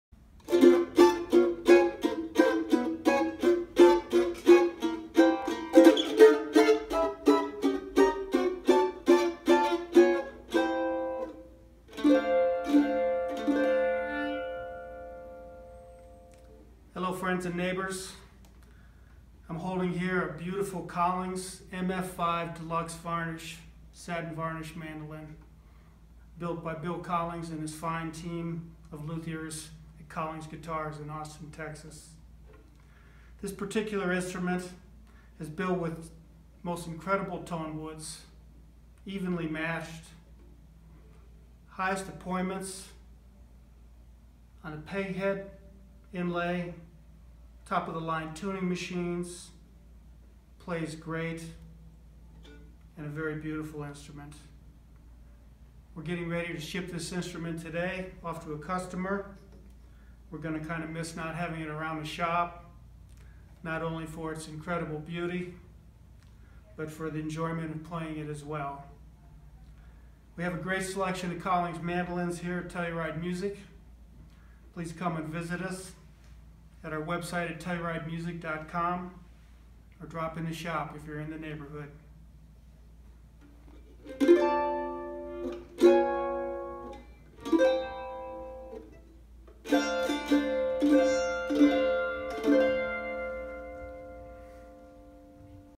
Collings Mandolins with Dave Lamb
Dave Lamb—owner of Telluride Music Co. from 1997-early 2017—sends off a killer Collings MF5 Deluxe Varnish that sold in late 2016. We love Collings mandolins at Telluride Music Co.